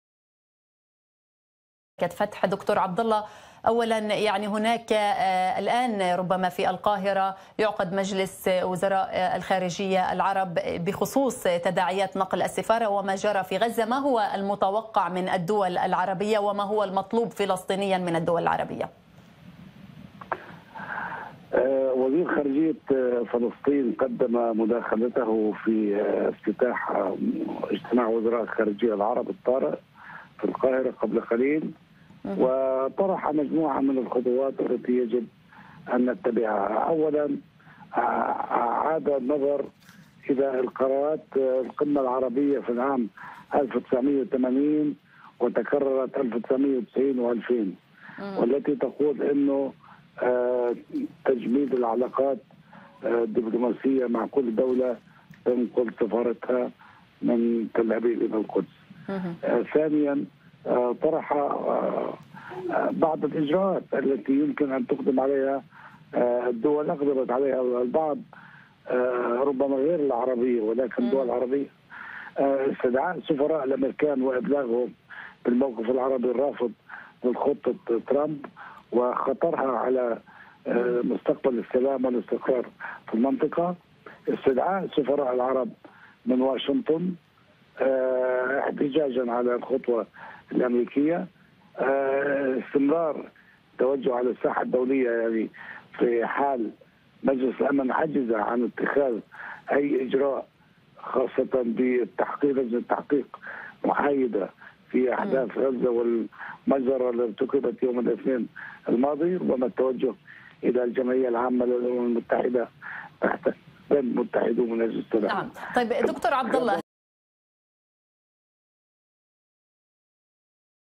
ما المتوقع من الدول العربية وما تتوخاه السلطة الفلسطينية منهم في القمة العربية؟ د. عبدالله يُجيب
أخبار الساعة | تقديم: شيرين يونس | ضيف: د. عبدالله عبدالله